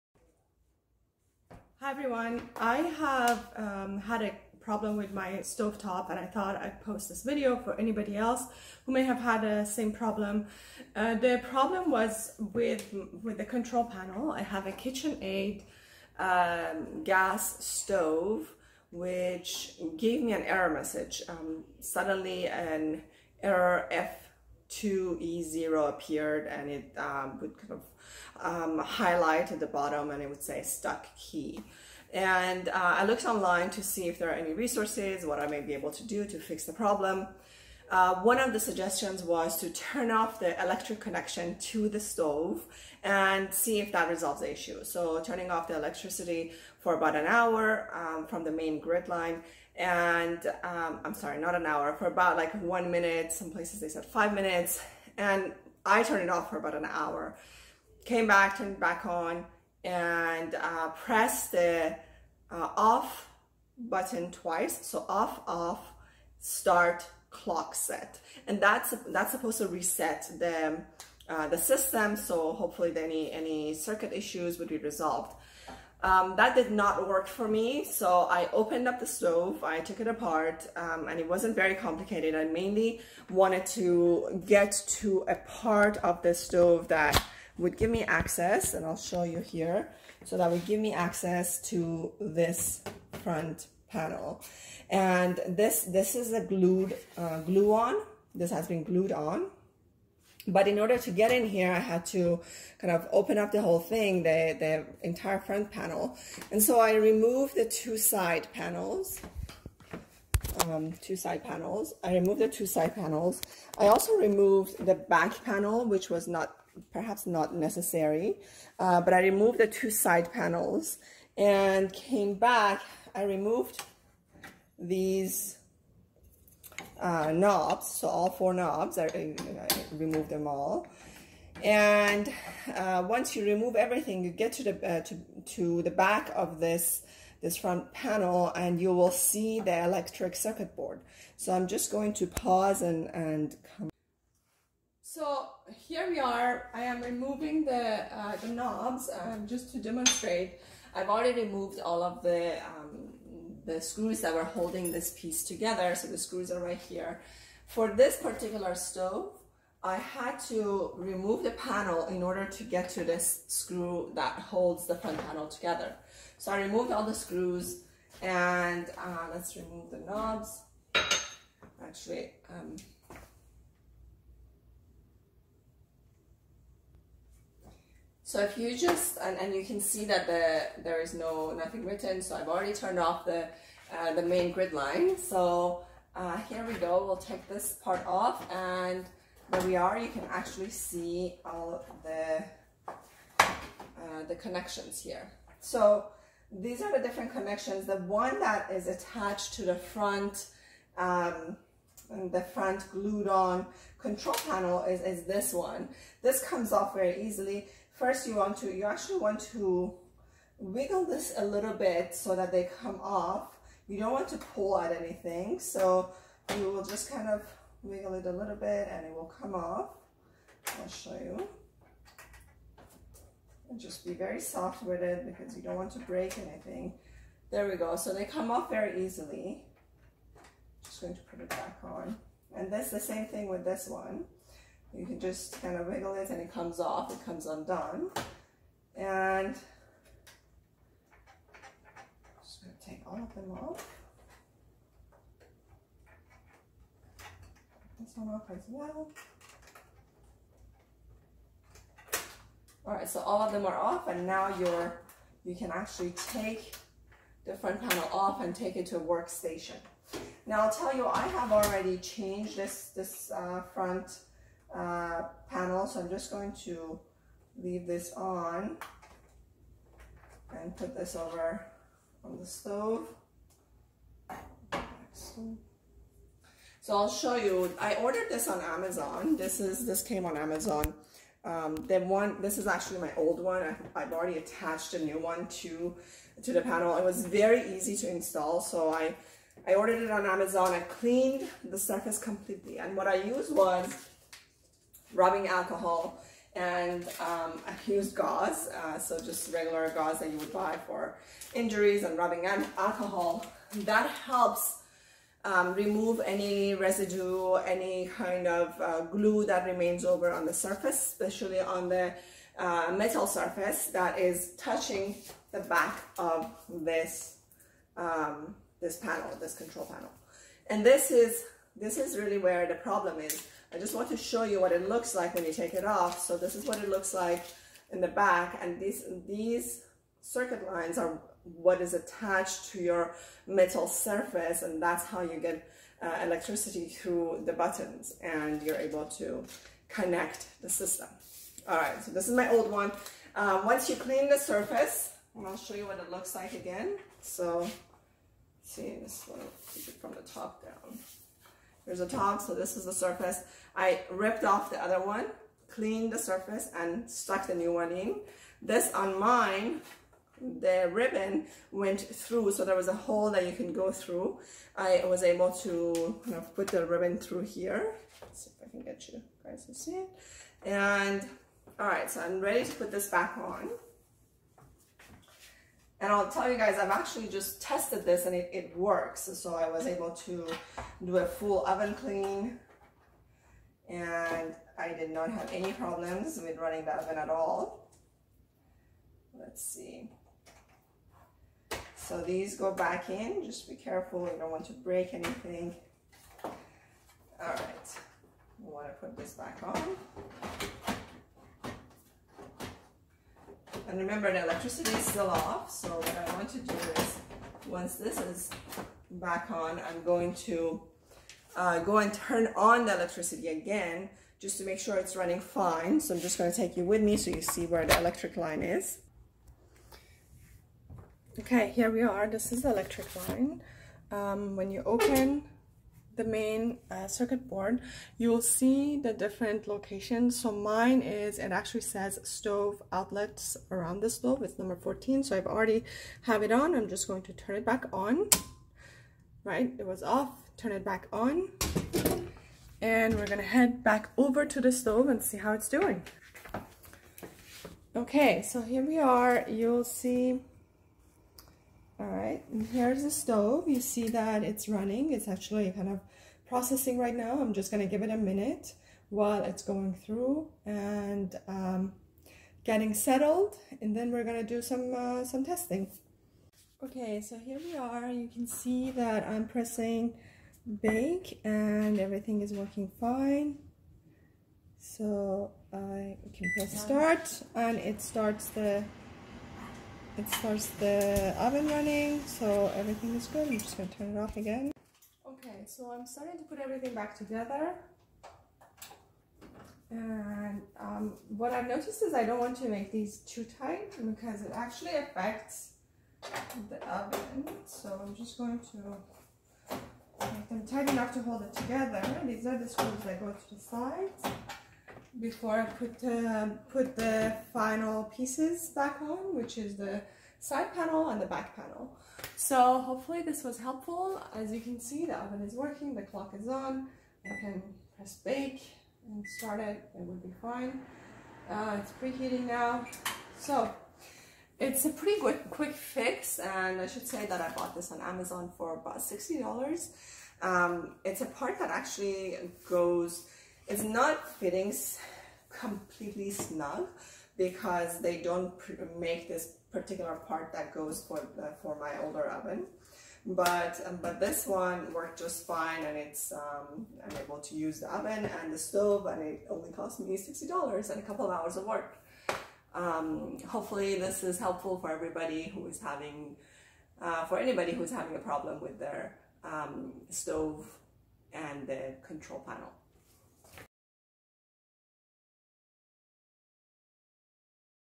KitchenAid/Whirlpool Gas Stove. Stuck Key, Err F2E0. Replace Button Control, Membrane Switch
This is not a typical video that I would post. However, since I did not see any women who had posted videos fixing home appliances, I wanted to demonstrate that fixing an appliance is gender-neutral.

Solutions for "Stuck Key" error. Replacing the glued on button control, membrane switch

If you are having problems with the button control not responding on Whirlpool/Kitchenaid gas stove.  Here are several easy options:
1) Unplug the stove or turn off the main electric power line to the stove for 1 minute. Re-connect the electricity, return to the stove and press the following keys in this order " Off" "Off" "Start" "Clock Set"

2) If the touchpad system does not reset using option 1 try replacing the switch membrane to solve your appliance problem.

Tools you need this repair/replacement:
1) #2 Philips Screw Driver
2) Rubbing alcohol
3) Towel for cleaning the surface

Get model-specific repair. Try contacting the company for troubleshooting and possible other solutions or parts that may be under warranty. If you have diagnosed that you need to replace the touchpad panel you can use this video. This was an easy and quick fix for me, so if you are willing to take a risk of changing the part yourself and you trust your skills for minimal work, this could be an easy fix for you as well.

Link to the item part- it is available through different sources- Prices ranges between $60-$180